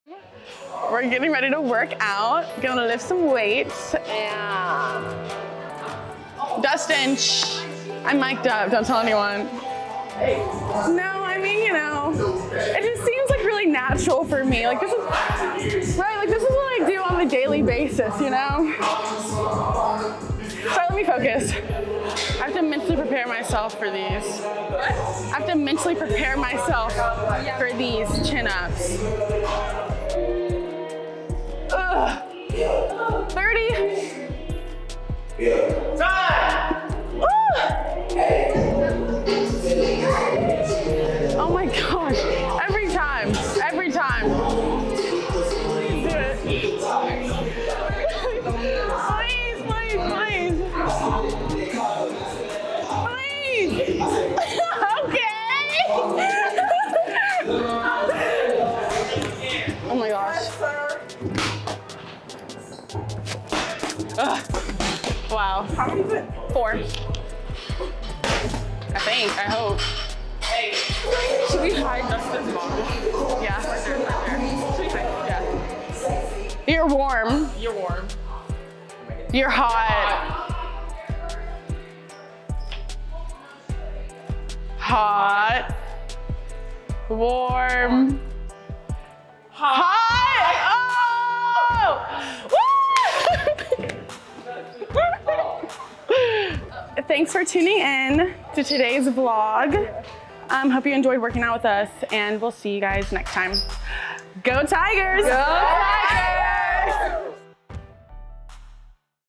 Sanaa Dotson Mic'd Up
Hear Sanaa Dotson live on the microphone during team weights!